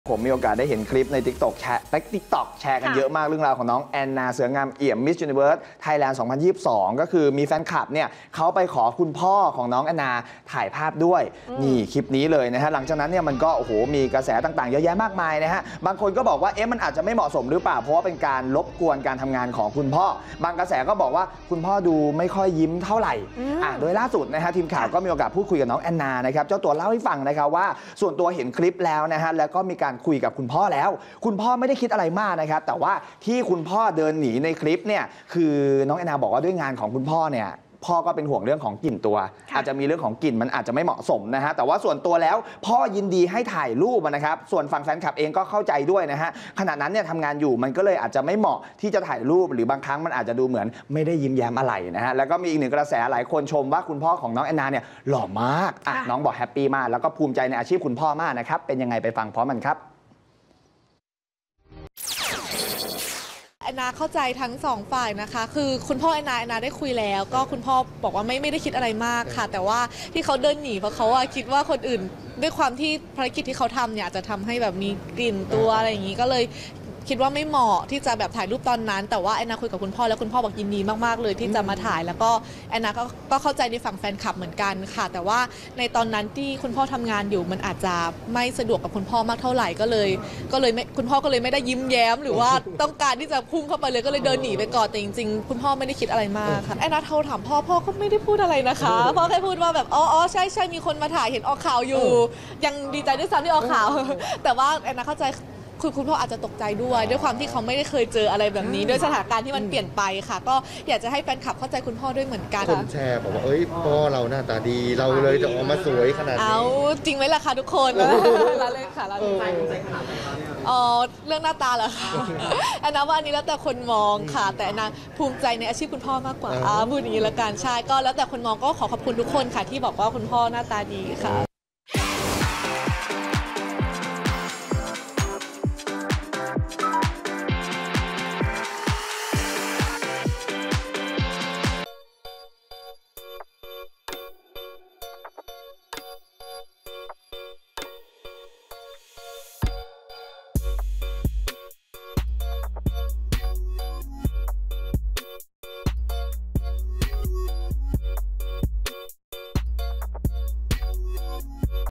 “แอนนา” เข้าใจแฟนคลับ เผยพ่อยินดีถ่ายรูปด้วย | Apop Today
เรียกได้ว่าพอมงลงก็มีกระแสเข้ามามากมายเลยสำหรับ “แอนนา เสืองามเอี่ยม” มิสยูนิเวิร์สไทยแลนด์ 2022 ที่ล่าสุดเกิดกระแสดราม่าถึงแฟนคลับที่ไปขอคุณพ่อของเธอถ่ายรูป ซึ่งมีบางคนมองว่าอาจจะไม่เหมาะสมและเป็นการไปรบกวนการทำงานของคุณพ่อ ล่าสุด “แอนนา” ให้สัมภาษณ์ถึงเรื่องนี้